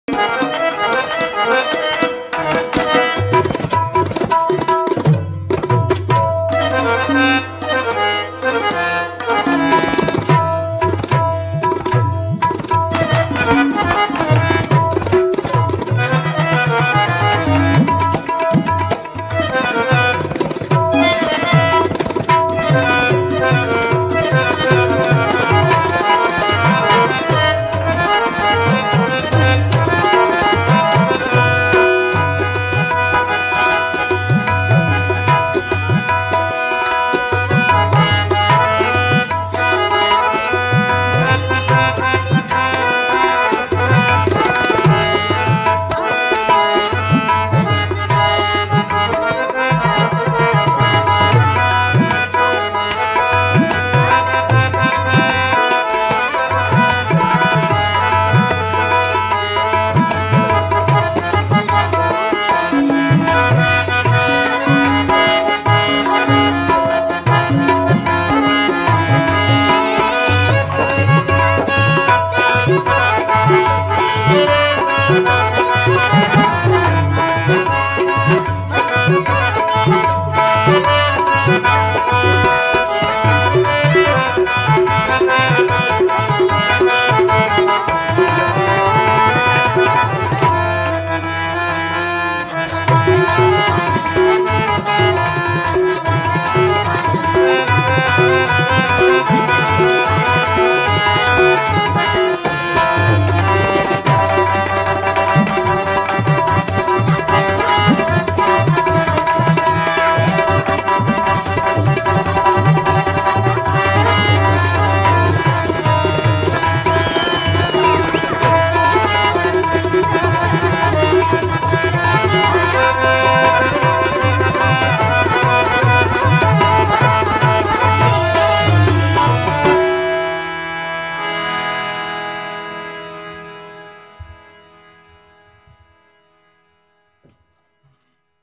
Anunad Present's a beautiful Presentation on Harmonium by Shruti Bhande.pl like, share.
This video provided by Ravi prakash Bhande for our channel anunad Thanks 🙏.soulfulmusic anupjalota anunad209# Shruti Bhande harmonium # Harmonium